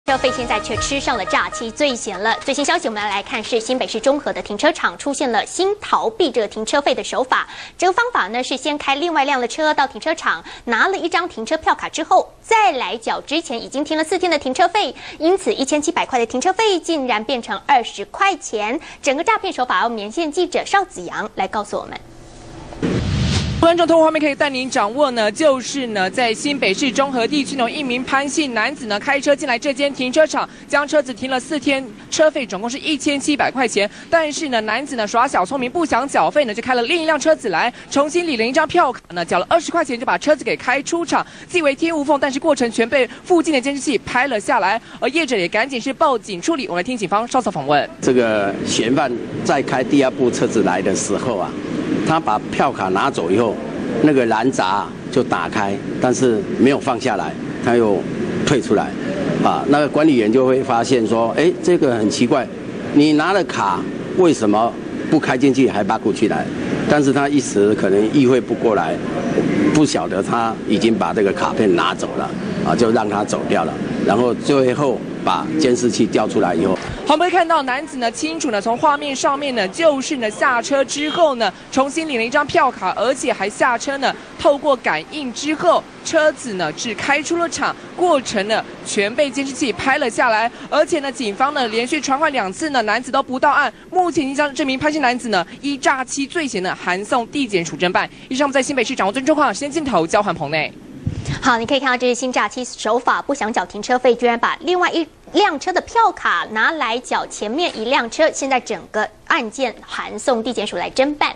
假聰明！不想繳停車費 乙車票開走甲車詐欺
中天新聞》耍小聰明卻用錯了地方，新北市中和地區某處停車場，3月11號一名男子開黑色轎車來停了4天，停車費總共1700元，但男子卻想省費用，開來另一輛紅色車子，感應拿到一張新停車票卡後，隨後離開，不久就拿著這張票卡繳了20元，把黑車子開出場，過程全被停車場監視器通通拍了下來。